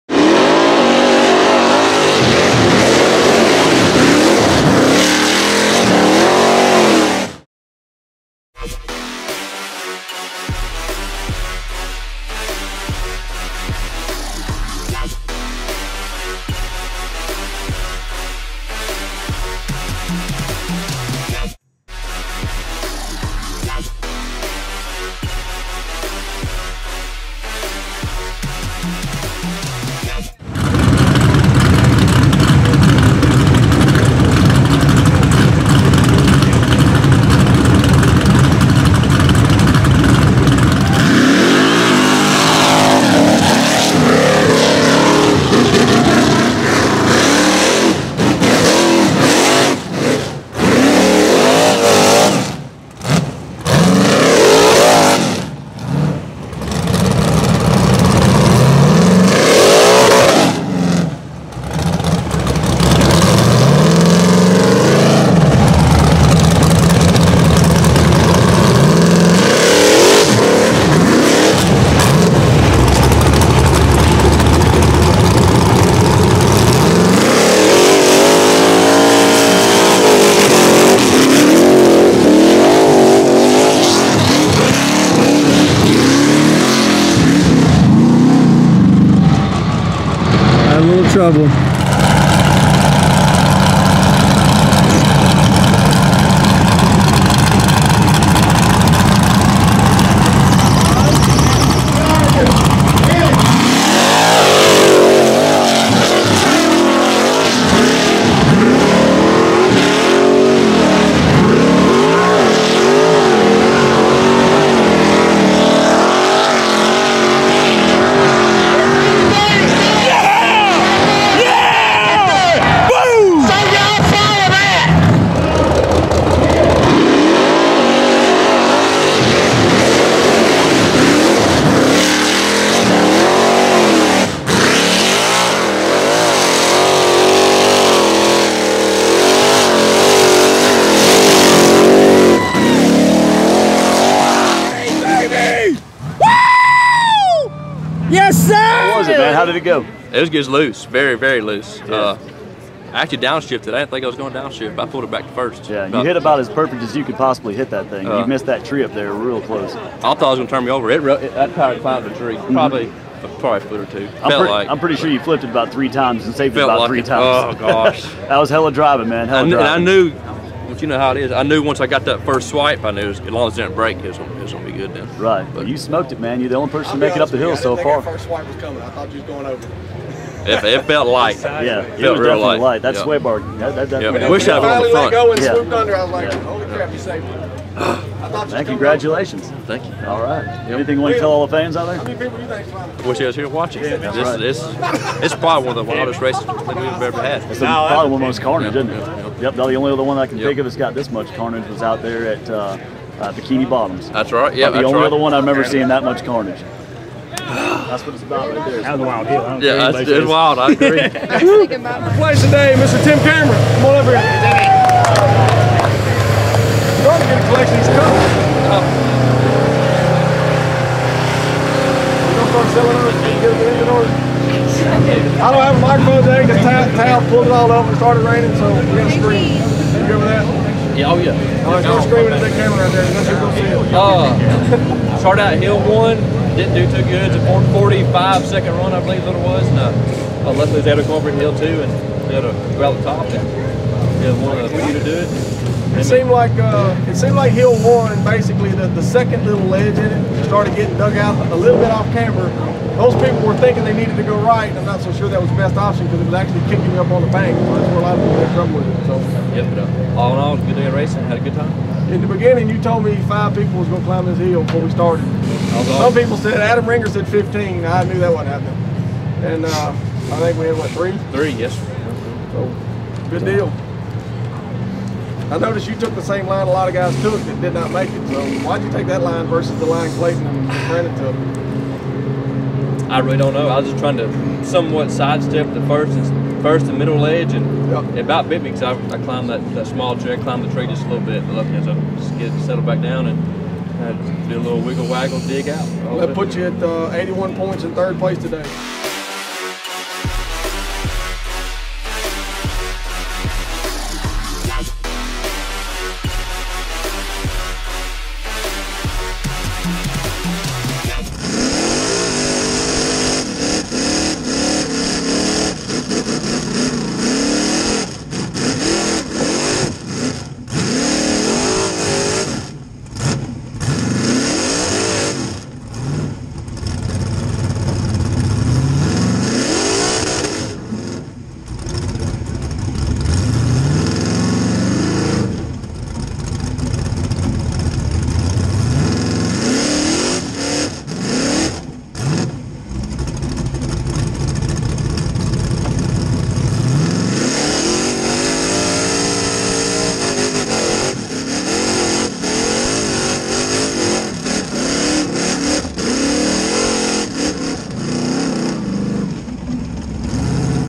TIM CAMERON FIRST PERSON TO EVER CLIMB SCREWDRIVER
Lots of people had tried and failed but Tim Cameron was the first person to climb the hill Screw Driver at Stony Lonesome Offroad Park.  Several guys crashed off the hill before TC got his shot but when it came down to it TC got the job done. TC was the 1st of only 3 drivers to successfully climb the hill and because of this he ended up in 3rd place for the day at the Southern Rock Race.